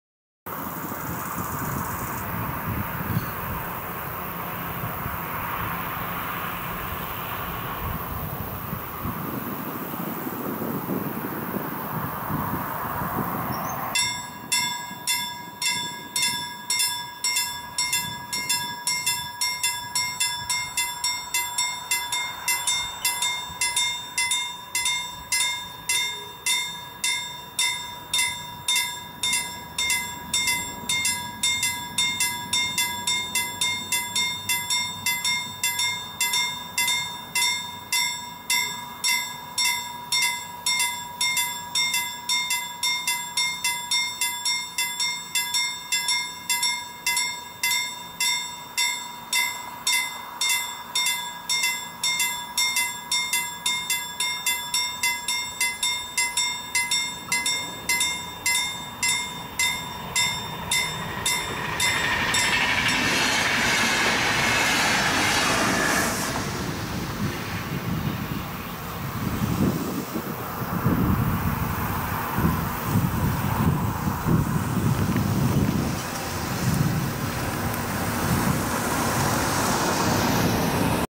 InterCity 465 passes Kaukolantie level crossing
Finnish InterCity 465 passes Kaukolantie level crossing on Vinkkilä, Sastamala, Finland.

InterCity 465 ohittaa Kaukolantien tasoristeyksen Vinkkilässä Sastamalassa.